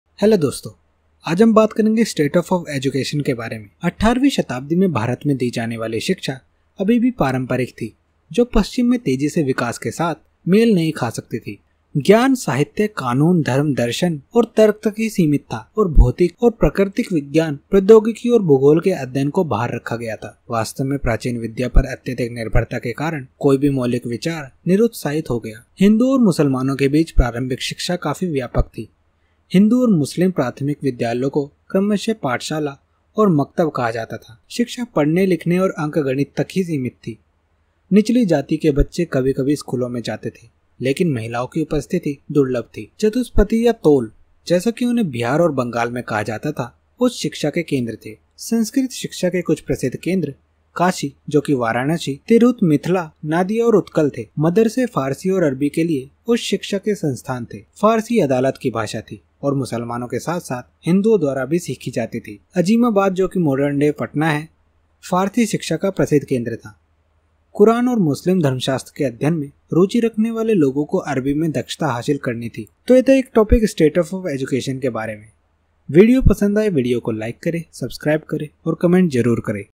Status of Education | Spectrum Modern History Chapter 4 | UPSC
Status of Education
The education imparted in 18th-century India was still traditional which could not match with the rapid developments in the West. The knowledge was confined to literature, law, religion, philosophy, and logic and excluded the study of physical and natural sciences, technology and geography. In fact, due to over-reliance placed on ancient learning, any original thought got discouraged. Elementary education among the Hindus and tne Muslims was quite widespread. The Hindu and Muslim elementary schools were called pathshalas and maktabs respectively. The education was confined to reading, writing, and arithmetic. Children from the lower caste sometimes attended the schools, but female presence was rare.
Chatuspathis or Tols, as they were called in Bihar and Bengal, were the centres of higher education. Some of the famous centres for Sanskrit education were Kasi (Varanasi), Tirhut (Mithila), Nadia and Utkala. Madrasahs were the institutions of higher learning for Persian and Arabic, Persian being the court language and learnt by the Muslims as well as the Hindus. Azimabad (Patna) was a famous centre for Persian education. People interested in the study of the Quran and Muslim theology had to acquire proficiency in Arabic.